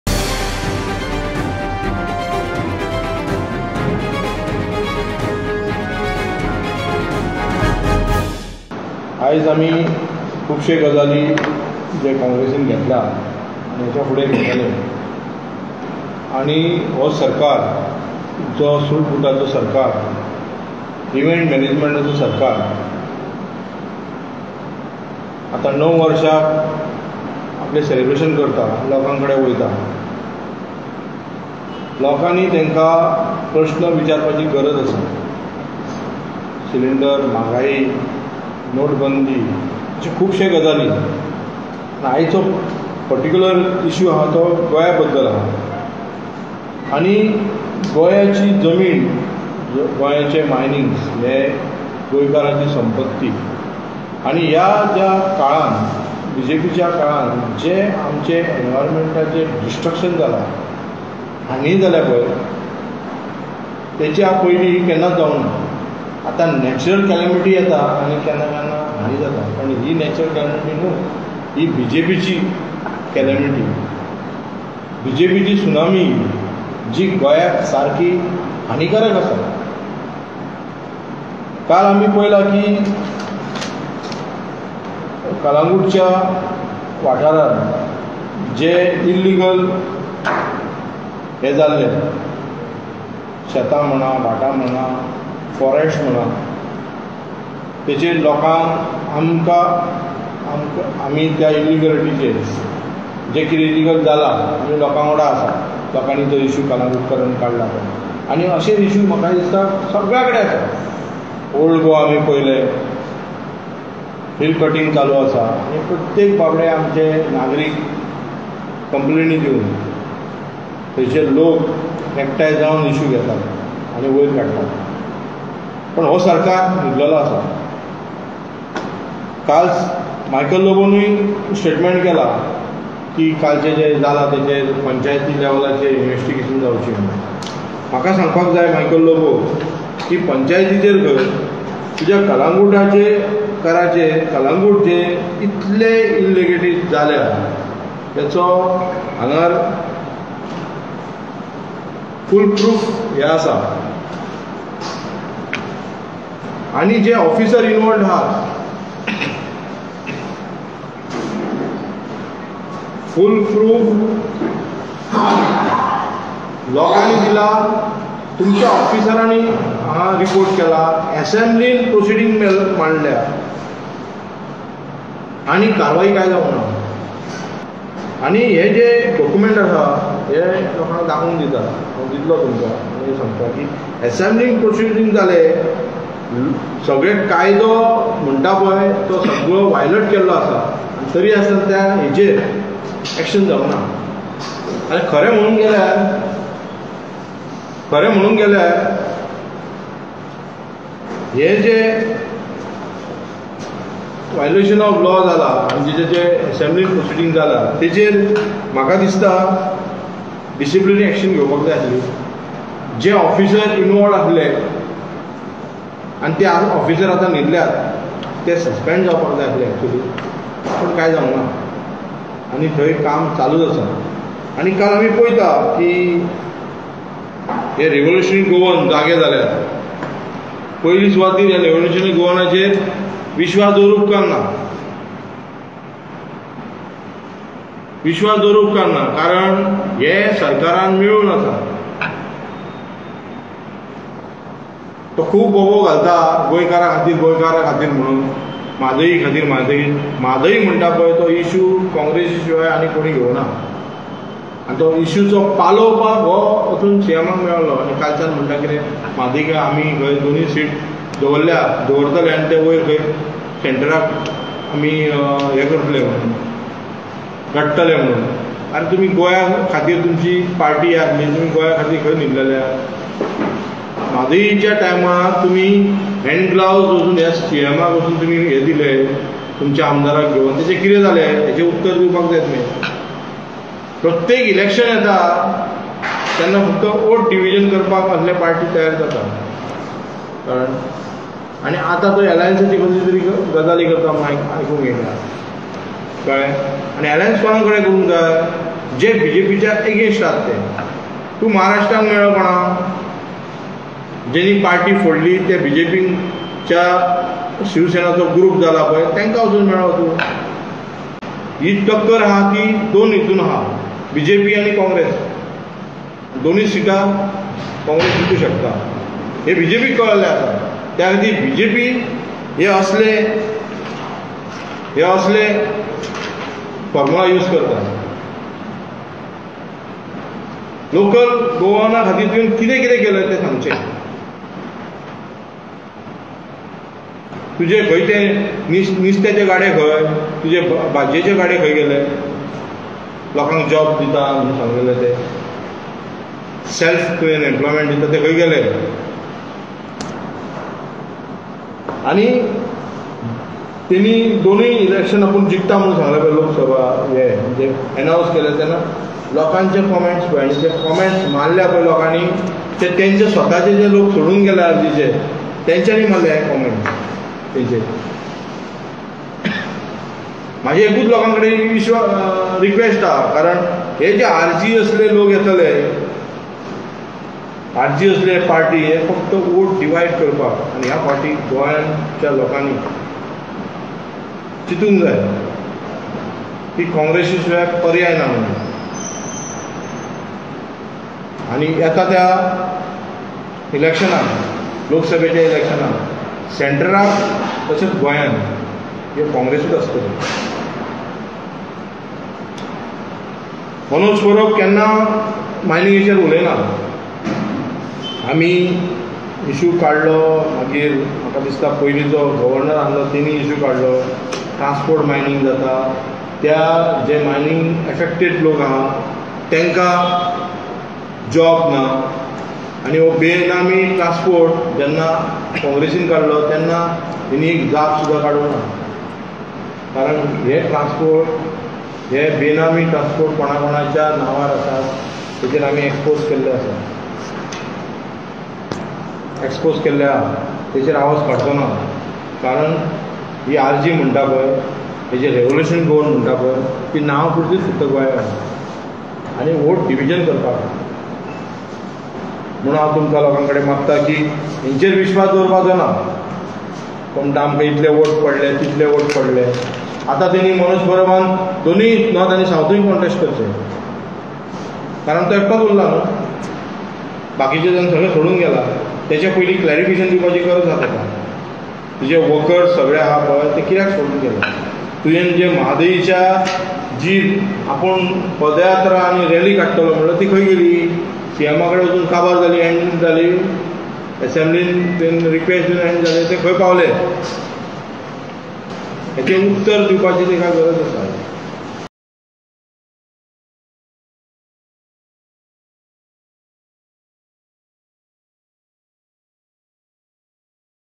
CONGRESS TAUNTS BJP
Says BJPs ‘Soot Boot Sarkar’ failed to give solace to people